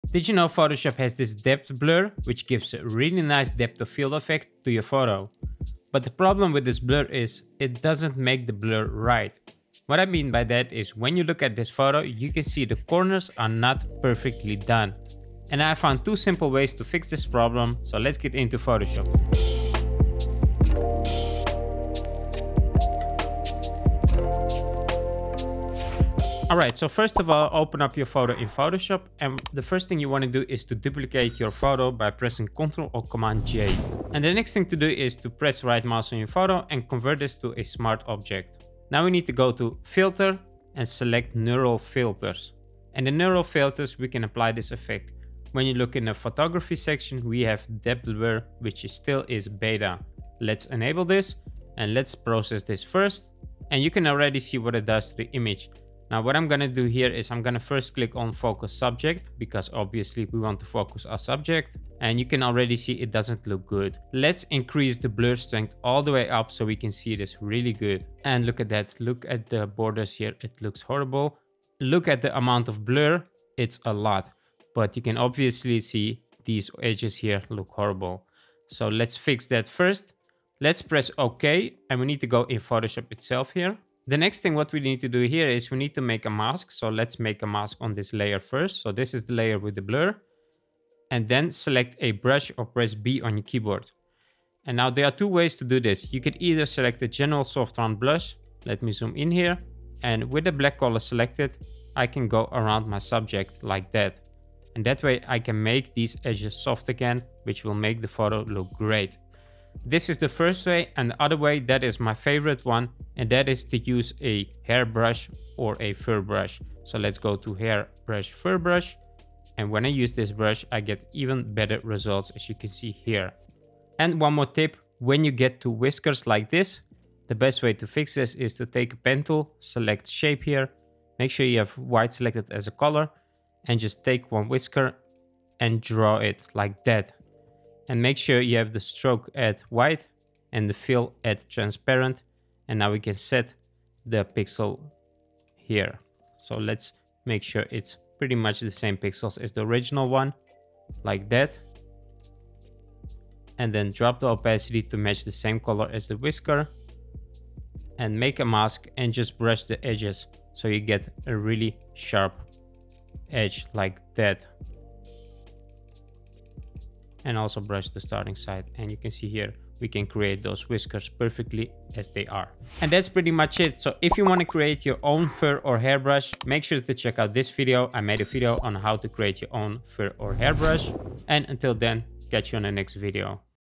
How to Use Lens Blur in Photoshop The Right Way!
Depth Blur in Adobe Photoshop is amazing but it has one major issue. It doesn't know how to make the borders of a subject correct.